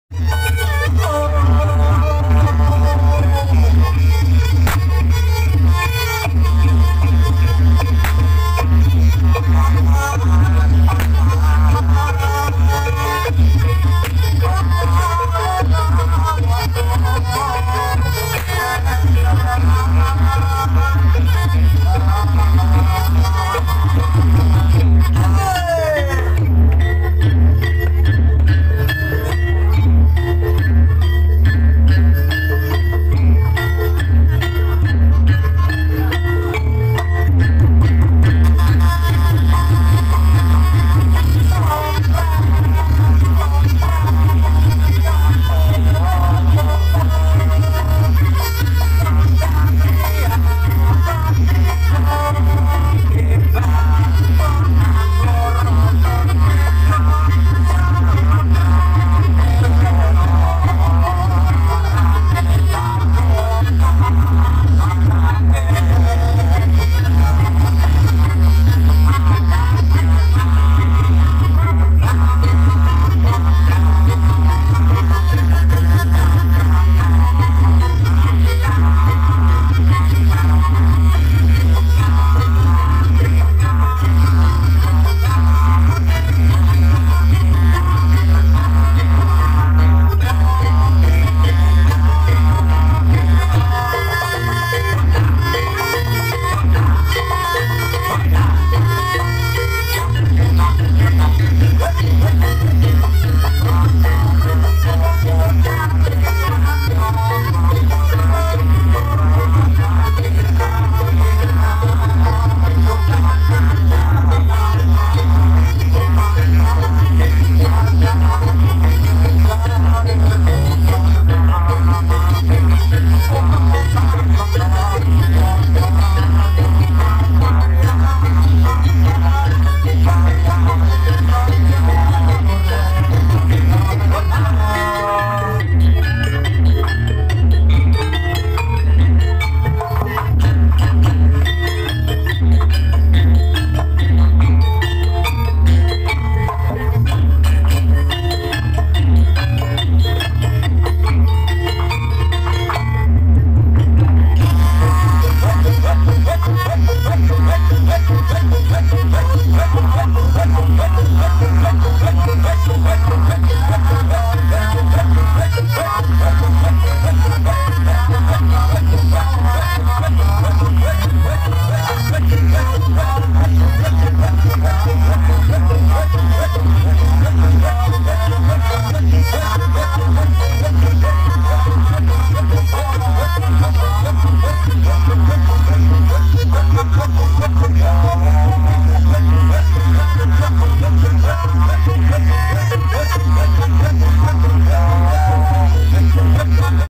MBABAR DI KEBON BARONGAN DEWA DEWI AL-HUDA DI DESA SAMPANG KARANGTENGAH DEMAK
Mbabar Di sawah Barongan Dewa Dewi Alhuda lokasi Sempit di Sampang Karang Tengah DEMAK

BARAYA START
Support Produk Seniman Lokal dan Budaya Jawa Tengah Indonesia 👌

Creators:
🏠 Jl.Dk Gebangsewu Mijen Demak